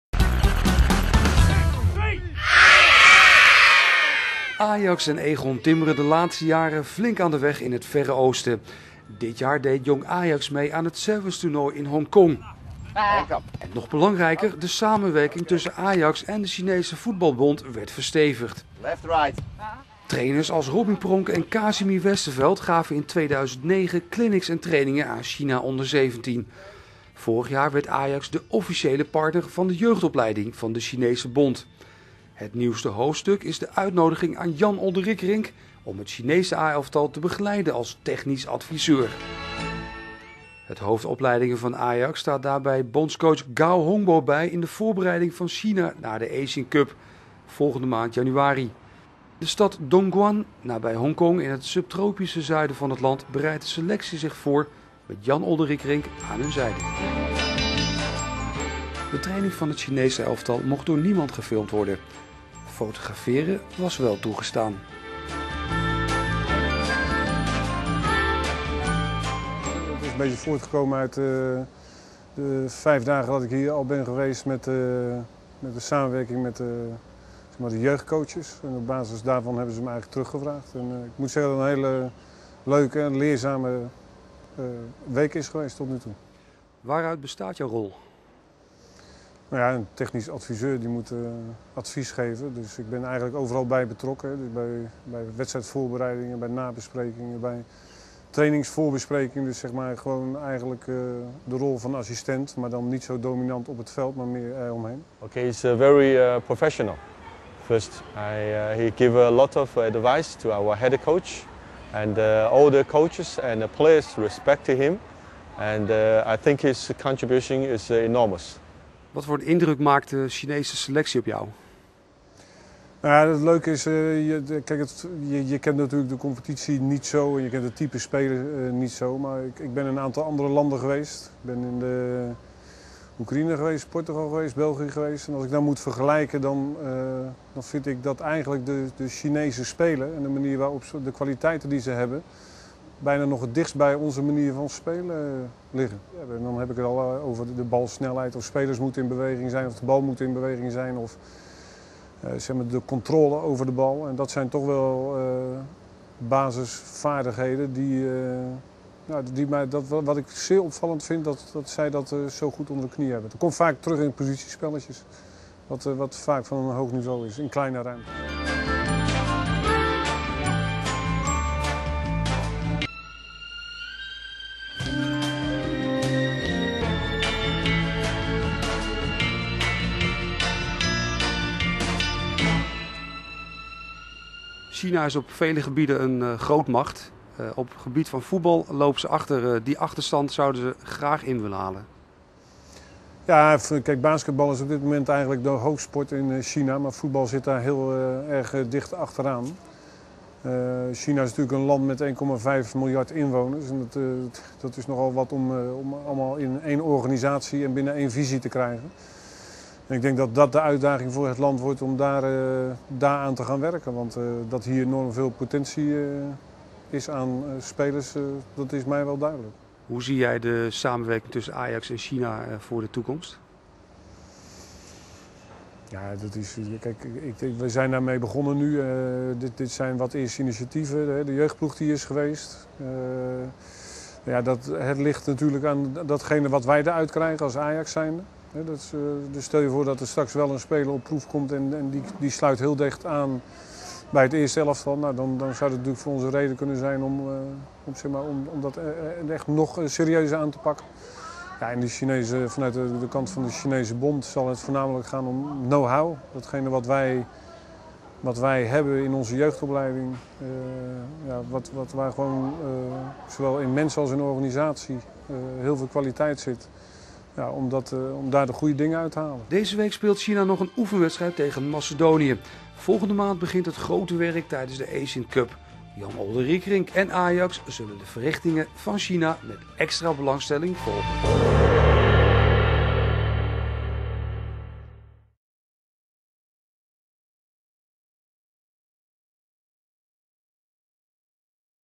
China blij met Ajax en Olde Riekerink
Jan Olde Riekerink is op dit moment in China waar hij als technisch adviseur de Chinese bondscoach Gao Hongbo bijstaat. Een eervolle opdracht bij het elftal dat volgende maand uitkomt in de Asian Cup.